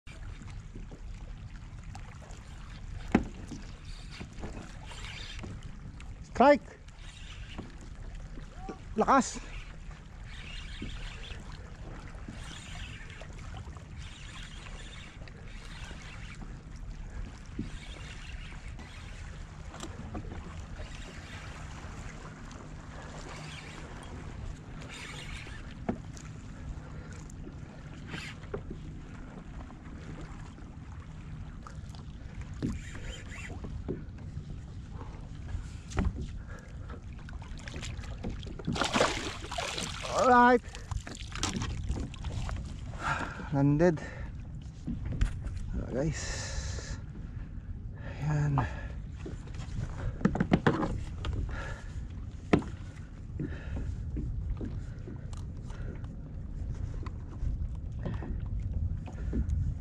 Hobie Outback Fishing
Fishing at the break waters with lots of groupers.

Salt and Freshwater fishing blogs and tutorials
God bless!